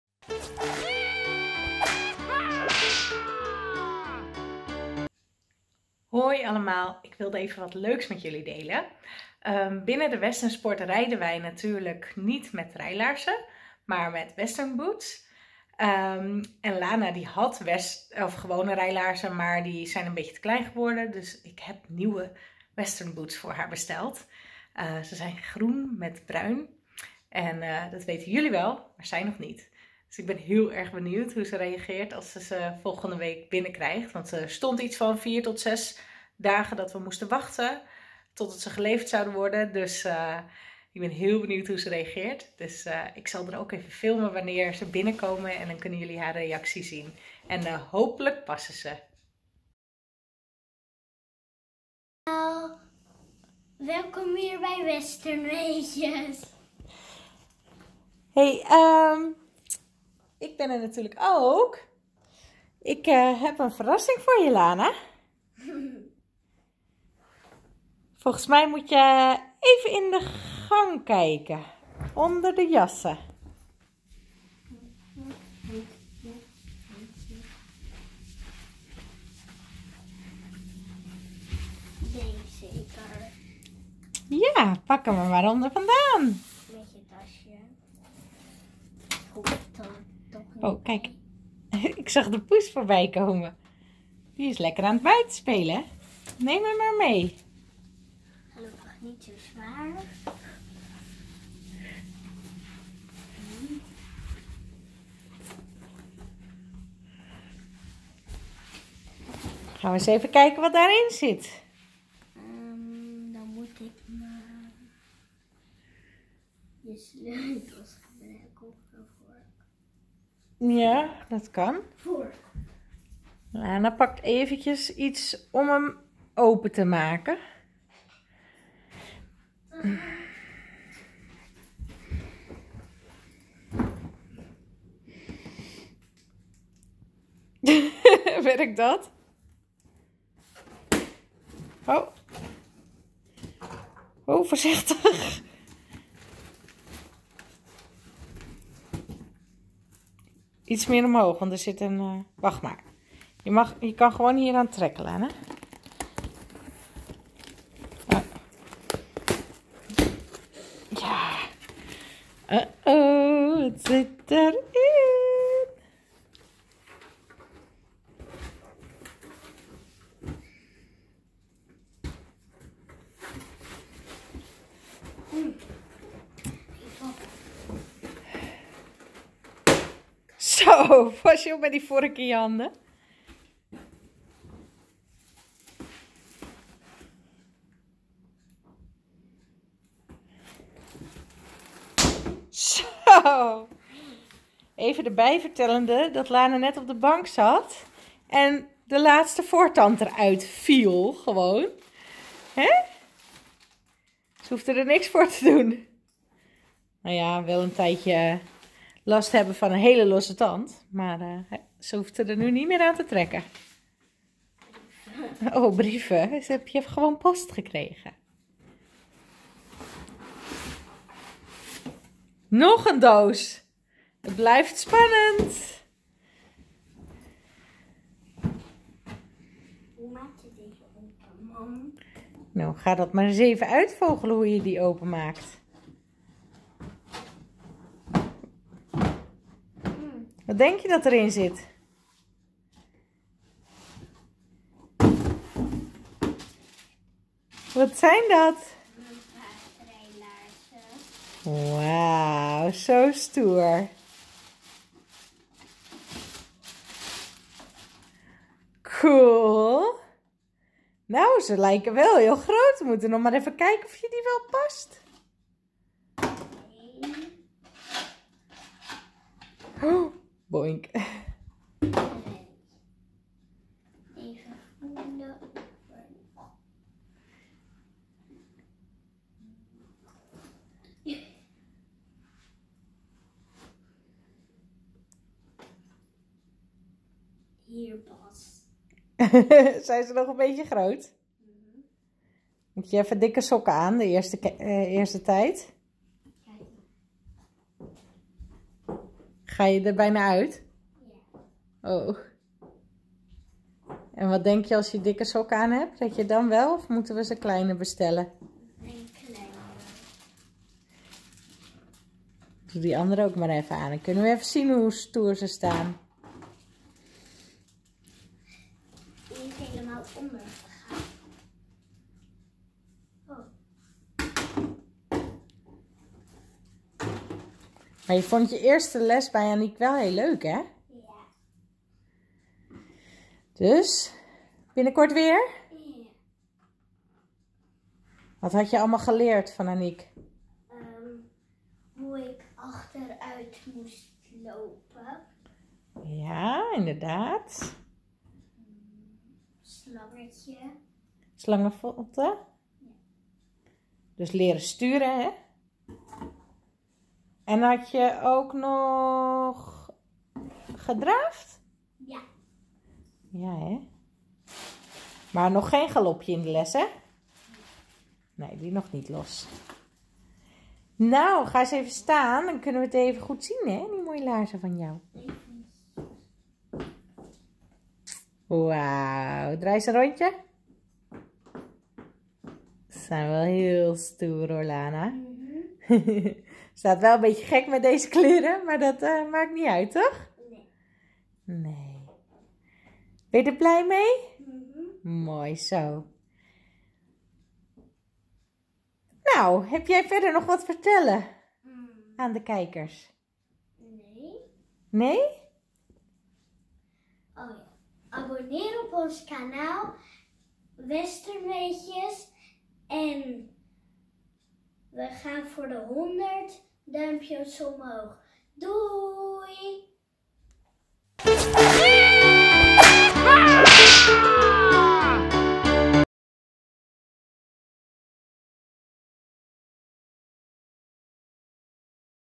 Verassing!!! Nieuwe Western Boots voor Lana!!! l WesternWeetjes
In de westernsport word er gereden met westernlaarzen. Lana had voorheen gewone dressuur laarzen. Inmiddels is ze hieruit gegroeid en toe aan nieuwe rijlaarzen. Zie in deze video hoe Lana verrast wordt!

Ben je trouwens enkel volger op Youtube en wil je nog meer kennis opdoen betreffende de Westernsport? Volg ons dan ook op Instagram en/of Facebook!